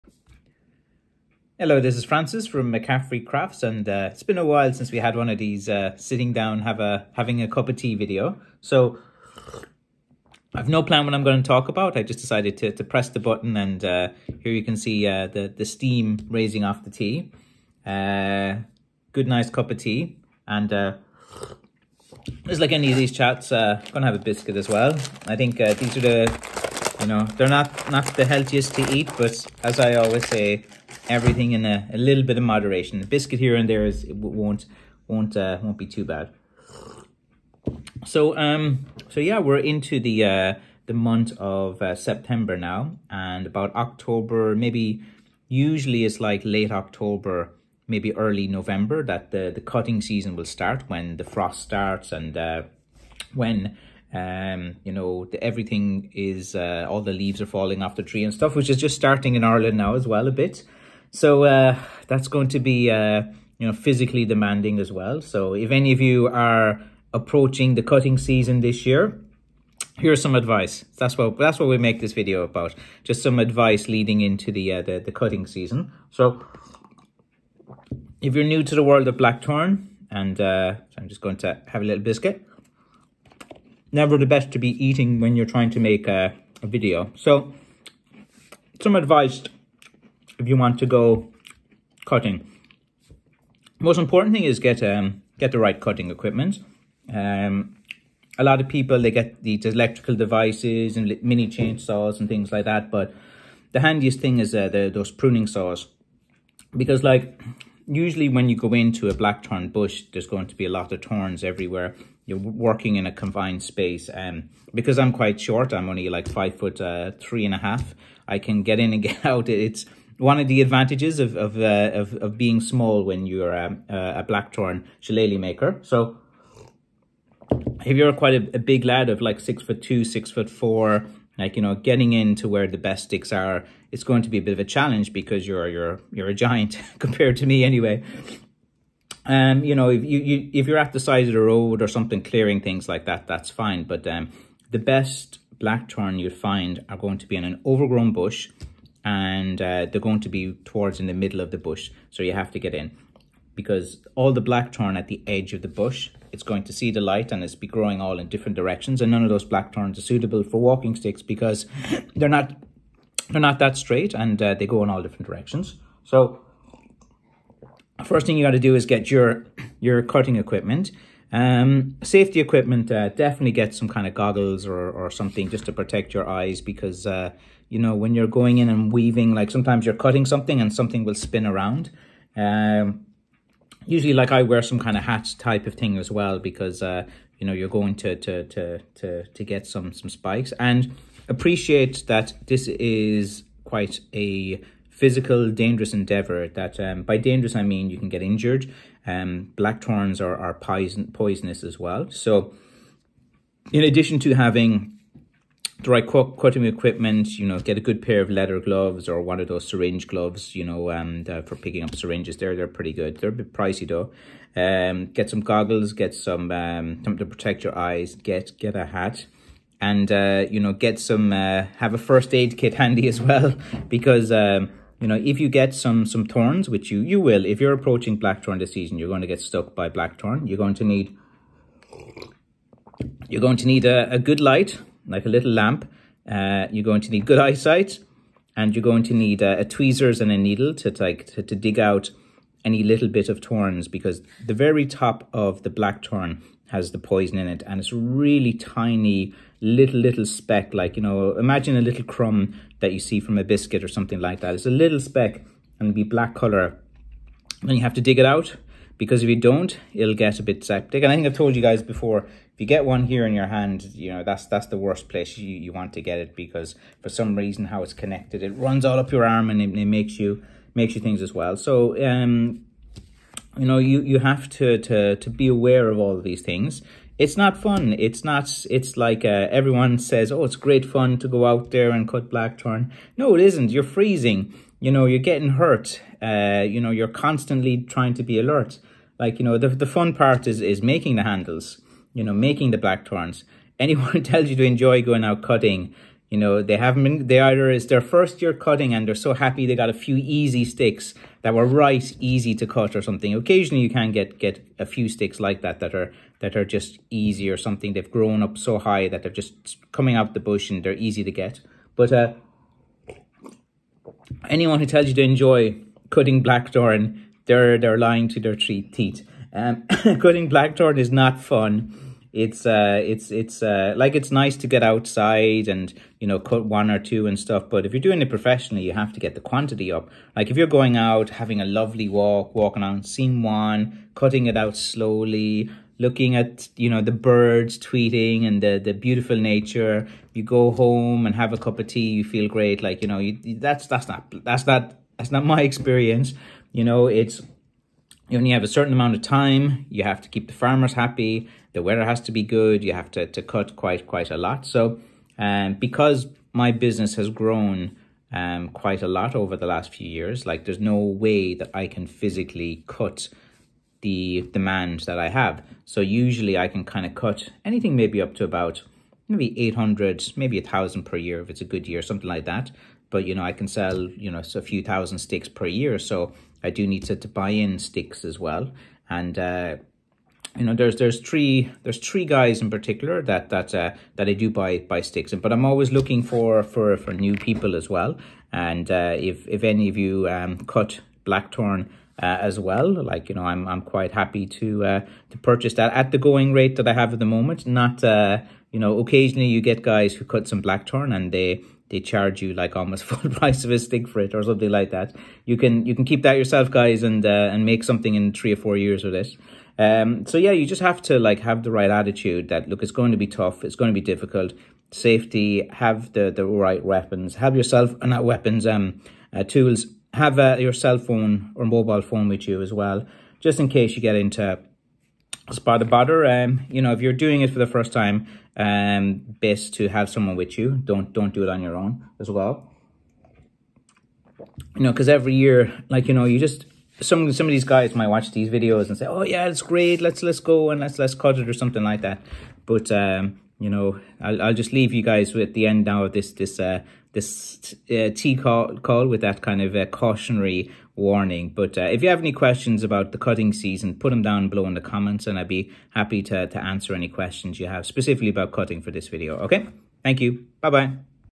First year cutting Blackthorn Wood - some advice from a full time shillelagh maker
3 day world wide deliver by courier with my blackthorn shillelagh walking sticks,

Livestream every Friday 6pm GMT on YouTube